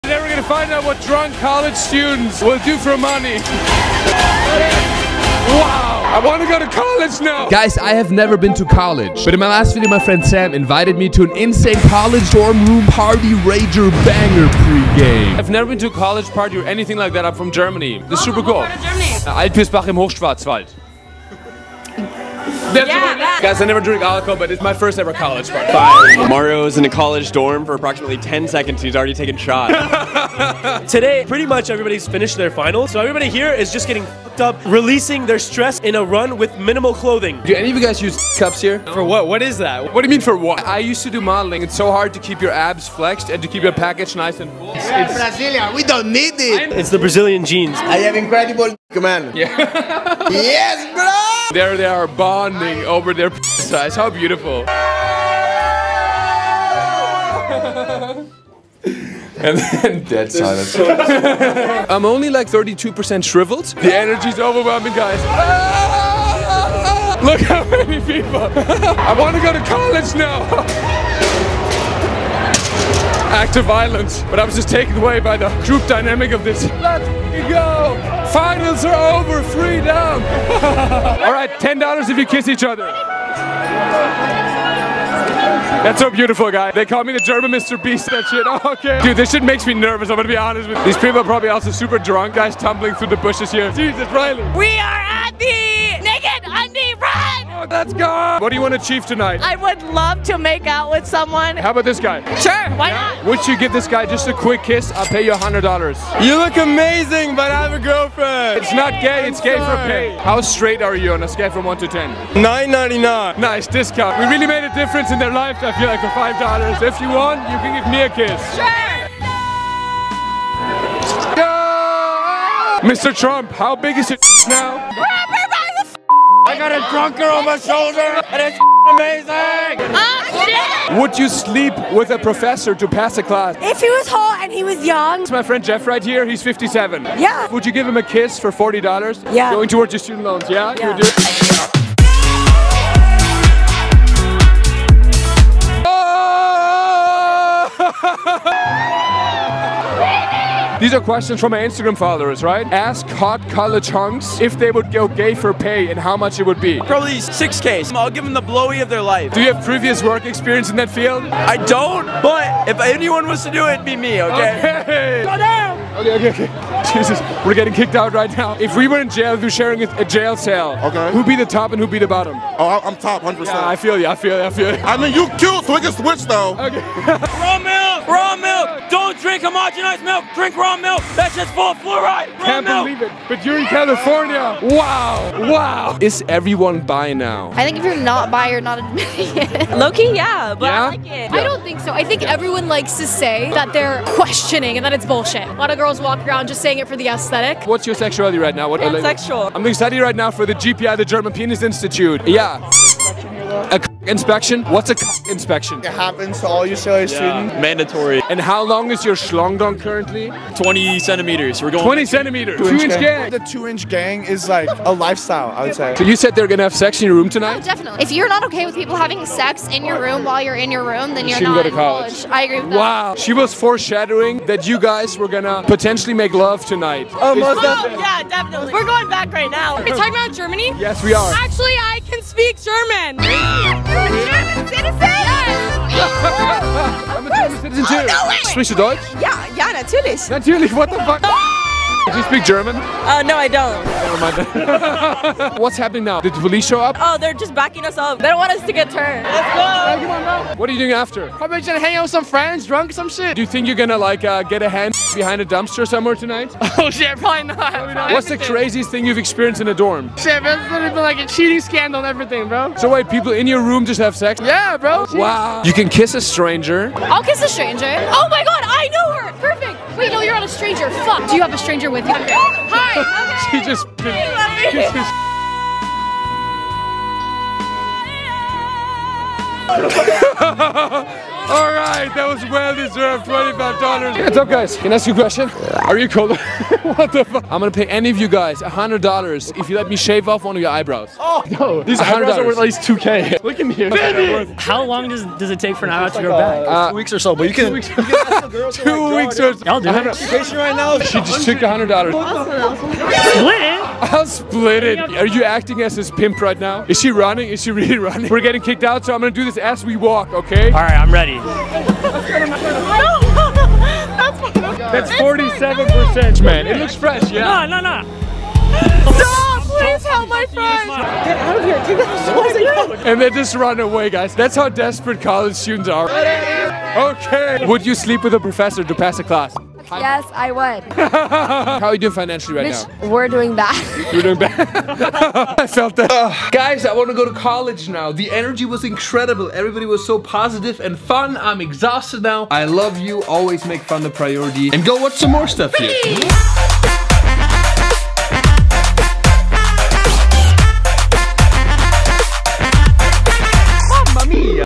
What DRUNK College Students do for ONE Dollar..
Interviewing Strangers at my First College Party at the UCLA Undie Run! 😂

Instagram: @marioadrion thanks @samibrielle and @sam_.f for the invite!

Itsamee Mario! 🥕
Model and Comedian